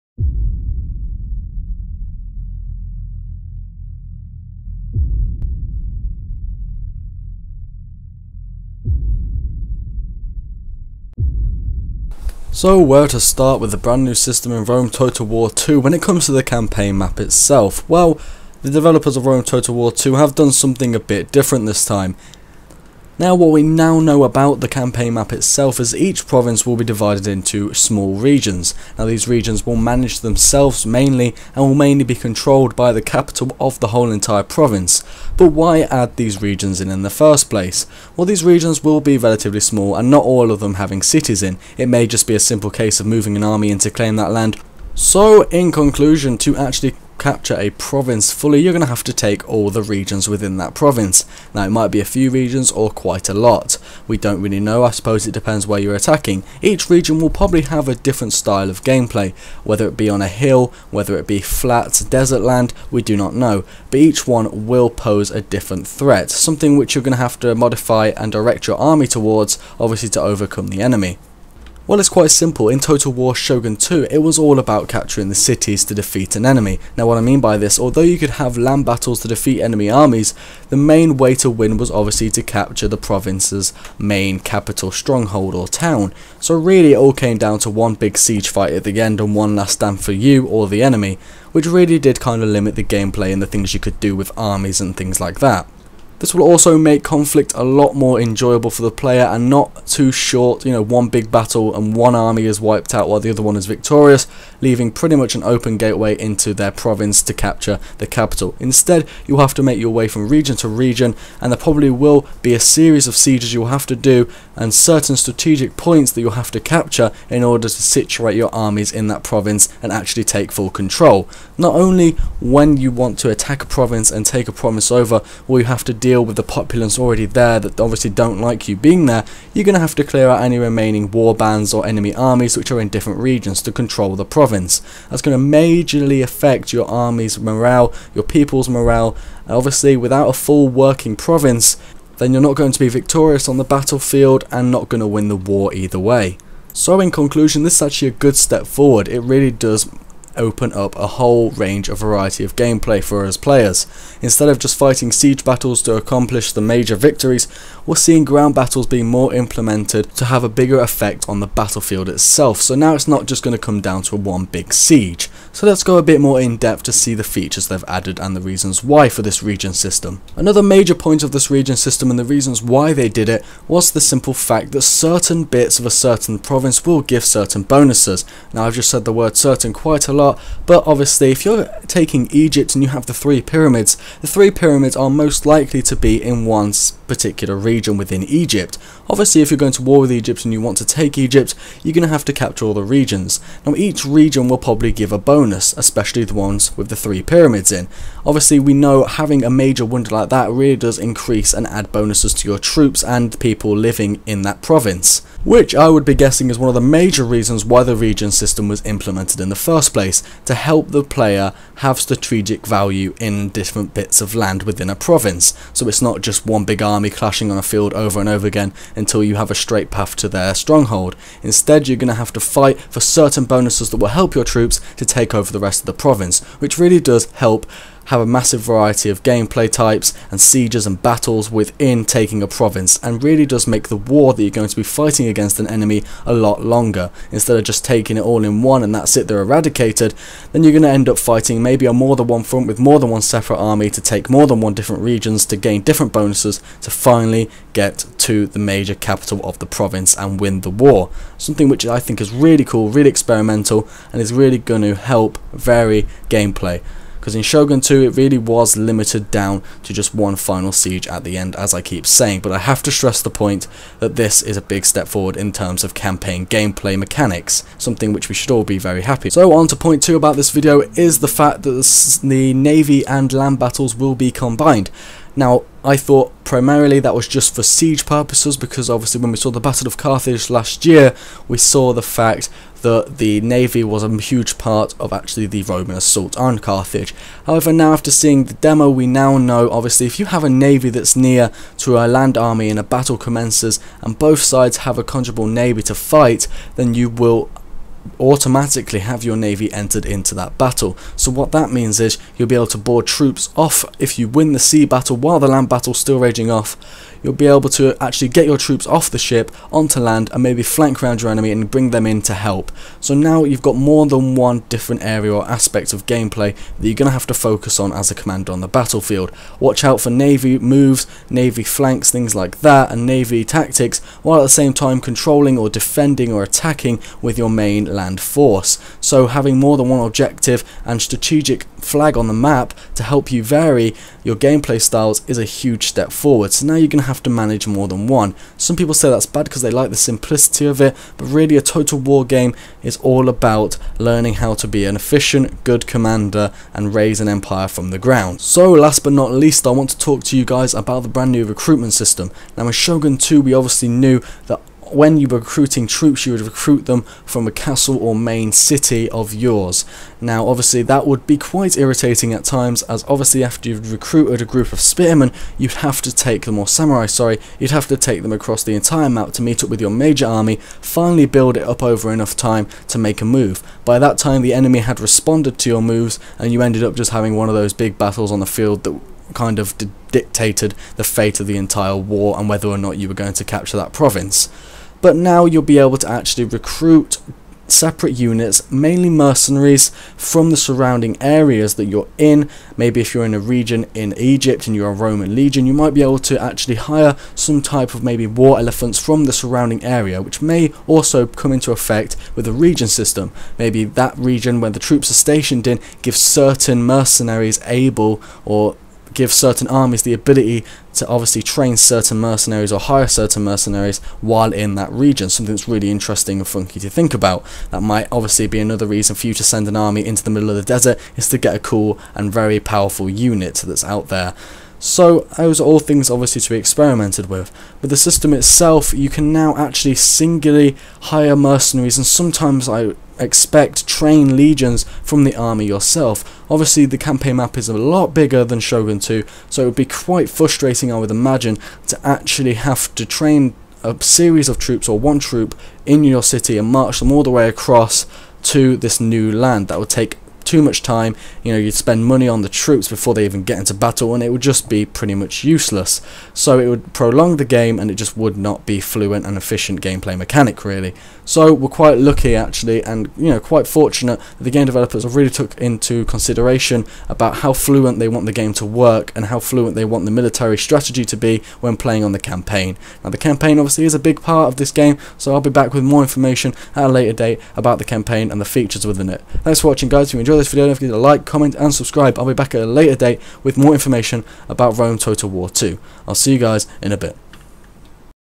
Rome Total War ll - Regions + Combined Battles Explained
hey guys and girls, just a vid telling you a bit about the combined battles and region system and how you can expect it to have a impact on you as the gamer when playing :)
havnt pre-ordered yet? do so now here!

or download?
GO STEAM! :D